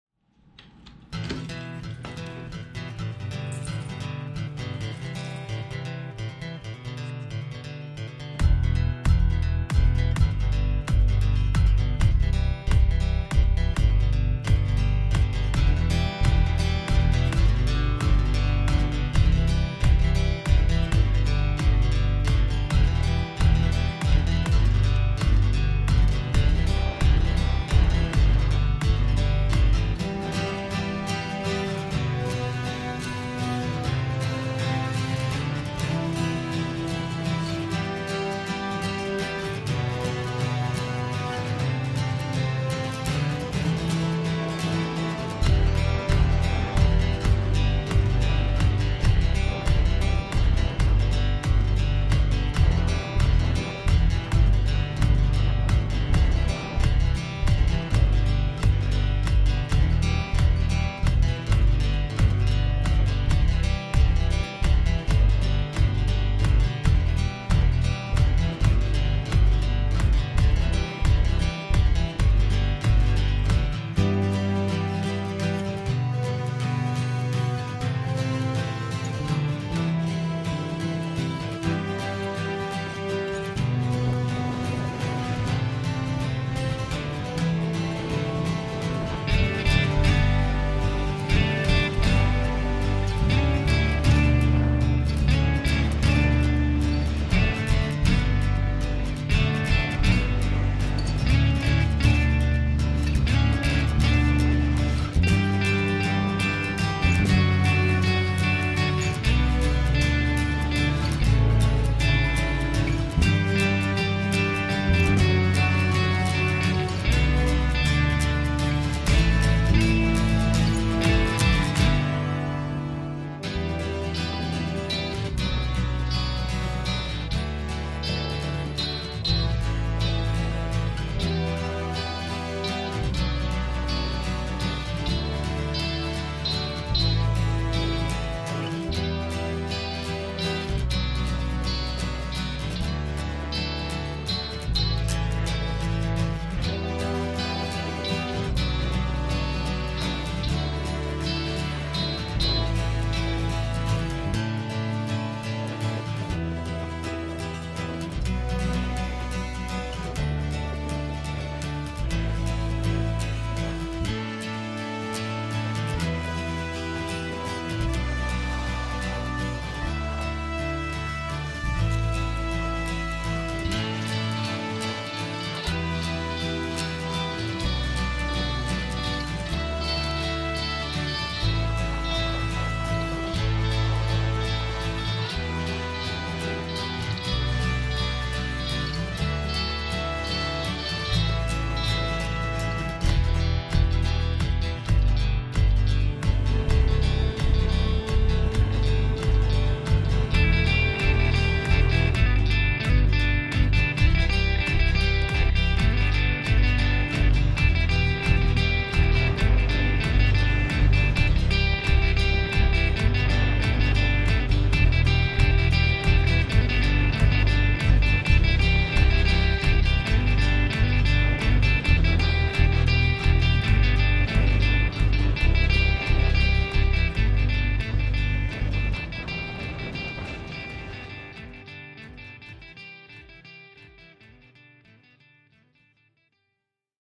Mont Ste Anne Quebec ski/snowboard trip March 2022
Snowboarding and Skiing Mont Ste Anne Quebec with Julie Genzel. We went on the hunt for powder before the season ended after getting skunked in Whistler back in February. Fantastic conditions for late march. Music written and recorded by myself, Gary Christie with a Martin acoustic guitar and Garageband software.